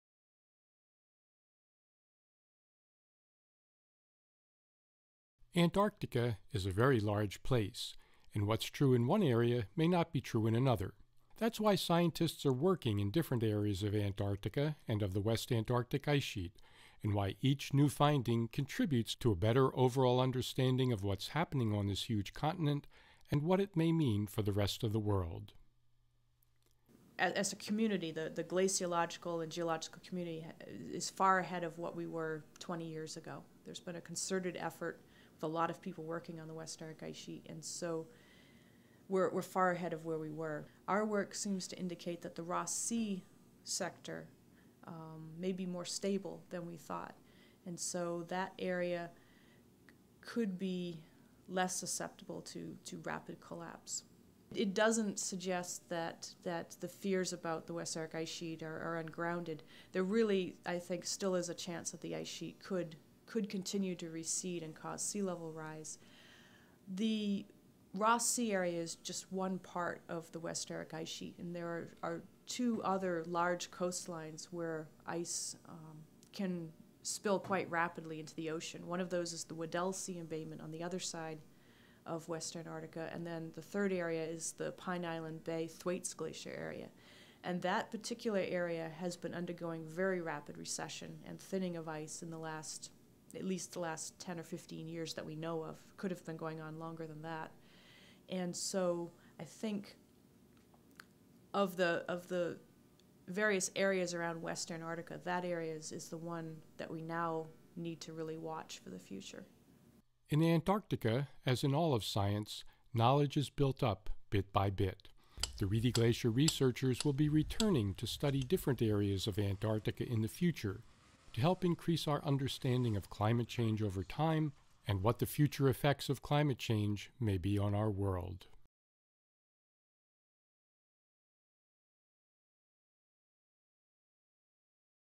What do we know, and what do we still need to learn?
Dr. Hall puts their findings in a broader context of what is known about ice sheets in other areas of Antarctica, and how it relates to wider concerns about the melting of ground-based ice sheets.